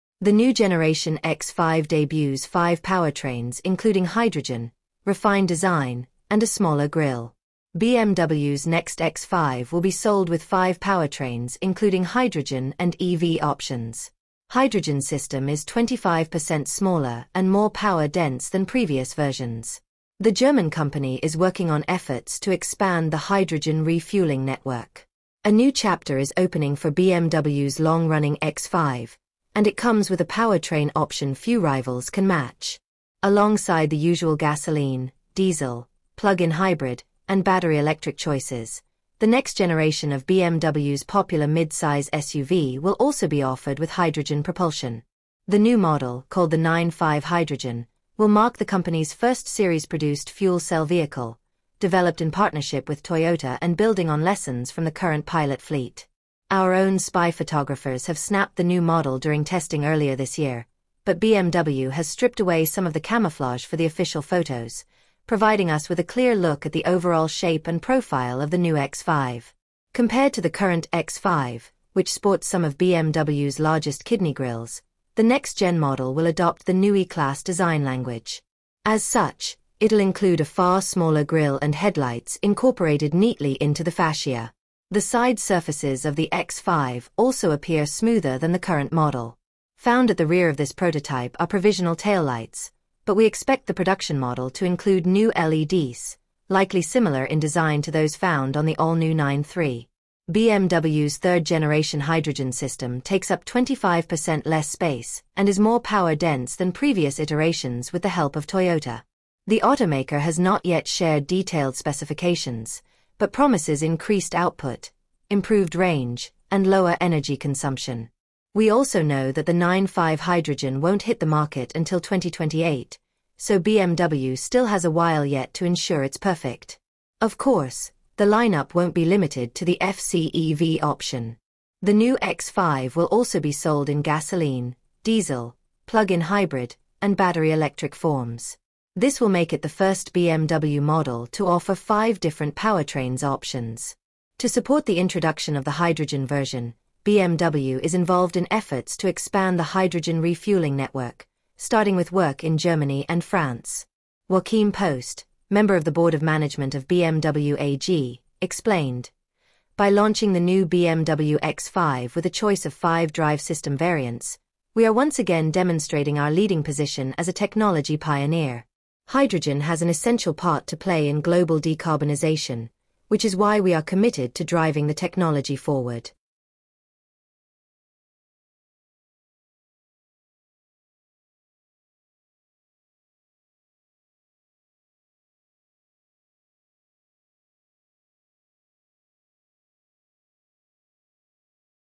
BMW’s Next X5 Will be Sold With Five Powertrains Including Hydrogen and EV Options.
The Next BMW X5 Could Finally Make Hydrogen Mainstream

Hydrogen system is 25 percent smaller and more power dense than previous versions.
The German company is working on efforts to expand the hydrogen refueling network.
A new chapter is opening for BMW’s long-running X5, and it comes with a powertrain option few rivals can match. Alongside the usual gasoline, diesel, plug-in hybrid, and battery-electric choices, the next generation of BMW’s popular mid-size SUV will also be offered with hydrogen propulsion.

Our own spy photographers have snapped the new model during testing earlier this year, but BMW has stripped away some of the camouflage for the official photos, providing us with a clear look at the overall shape and profile of the new X5.

Compared to the current X5, which sports some of BMW’s largest kidney grilles, the next-gen model will adopt the Neue Klasse design language. As such, it’ll include a far smaller grille and headlights incorporated neatly into the fascia.

The side surfaces of the X5 also appear smoother than the current model. Found at the rear of this prototype are provisional taillights, but we expect the production model to include new LEDs, likely similar in design to those found on the all-new iX3.

BMW’s third-generation hydrogen system takes up 25 percent less space and is more power dense than previous iterations with the help of Toyota. The automaker has not yet shared detailed specifications, but promises increased output, improved range, and lower energy consumption. We also know that the iX5 Hydrogen won’t hit the market until 2028, so BMW still has a while yet to ensure it’s perfect.

Of course, the lineup won’t be limited to the FCEV option. The new X5 will also be sold in gasoline, diesel, plug-in hybrid, and battery-electric forms. This will make it the first BMW model to offer five different powertrains options.

To support the introduction of the hydrogen version, BMW is involved in efforts to expand the hydrogen refueling network, starting with work in Germany and France.

Joachim Post, Member of the Board of Management of BMW AG, explained: “By launching the new BMW X5 with a choice of five drive system variants, we are once again demonstrating our leading position as a technology pioneer. Hydrogen has an essential part to play in global decarbonisation, which is why we are committed to driving the technology forward.”

Source: Carscoops